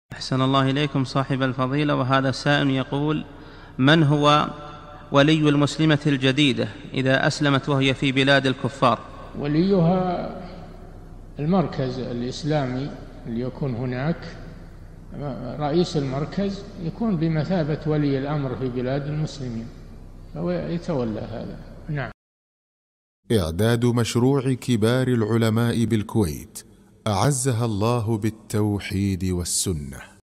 [156 -560] من هو ولي المسلمة الجديدة إذا أسلمت وهي في بلاد الكفار؟ - الشيخ صالح الفوزان
-"السؤال: من هو ولي المسلمة الجديدة إذا أسلمت وهي في بلاد الكفار؟
وليها المركز الإسلامي الذي يكون هناك، رئيس المركز يكون بمثابة ولي الأمر في بلاد المسلمين ويتولى هذا.
للاشتراك في قناة الشيخ صالح الفوزان
سؤال وجواب من شرح السنة للبربهاري ( 560 ) مقطع

للاستفسار والإقتراحات والدعم
رئيس الفريق
لمشروع كبار العلماء